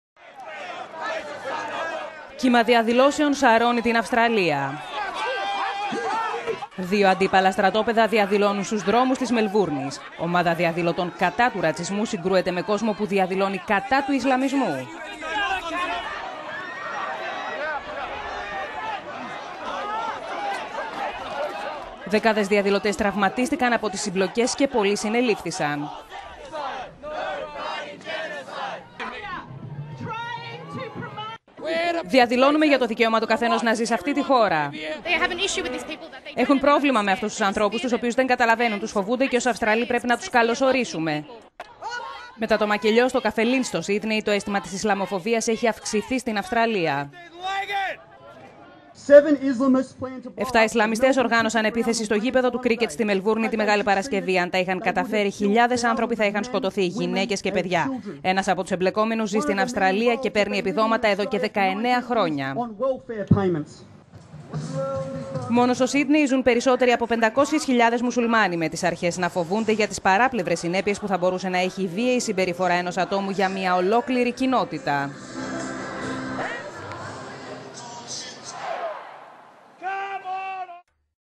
Αυστραλία: Συμπλοκές μεταξύ διαδηλωτών
Κύμα διαδηλώσεων σαρώνει την Αυστραλία.

Δύο αντίπαλα στρατόπεδα διαδηλώνουν στους δρόμους της Μελβούρνης.

Ομάδα διαδηλωτών κατά του ρατσισμού συγκρούεται με κόσμο που διαδηλώνει κατά του ισλαμισμού.

Δεκάδες διαδηλωτές τραυματίστηκαν από τις συμπλοκές και πολλοί συνελήφθησαν.

«Είμαστε εδώ για να διαδηλώσουμε για το δικαίωμα του καθενός να ζει σε αυτή τη χώρα» είπε ένας διαδηλωτής.

Μετά το μακελειό στο καφέ Λίντ το αίσθημα της ισλαμοφοβίας έχει αυξηθεί στην Αυστραλία. Το Δεκέμβριο ένοπλος ει…